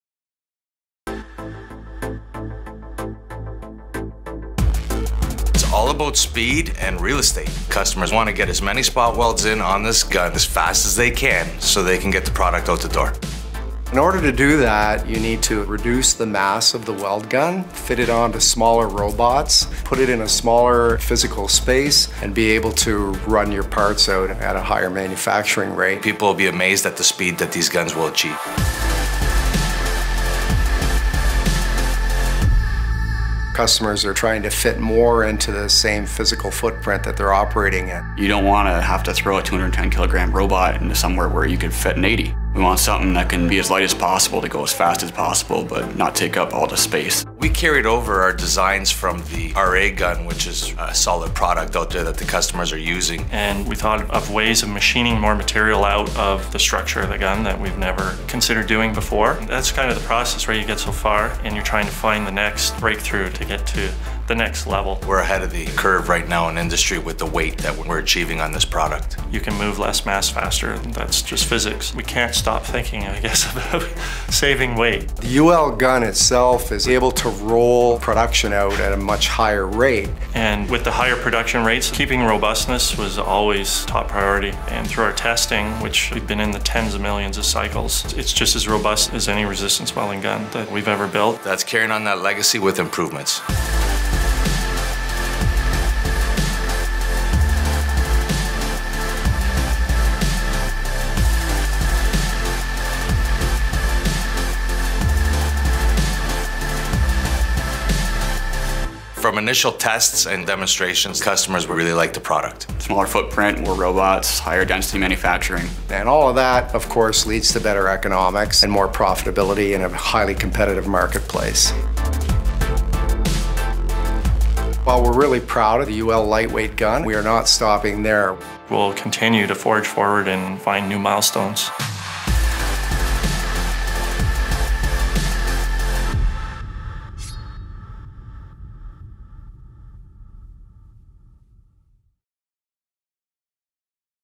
CenterLine FlexGun™ UL 2019
The FlexGun™ UL is an ultra-lightweight weld gun that is part of CenterLine’s FlexGun family of gun packages. With over 300 available models in X, C and Pinch configurations, and a vast majority of models with total gun mass under 70kg, the FlexGun UL is ideal for use on robot models with payloads starting at 80kg and for applications requiring between 2.5 to 8.3 kN of weld force.

With integrated robot mounting, superior strength to weight ratio construction, and ultra-compact size, the FlexGun UL is the right choice for high-density production needs.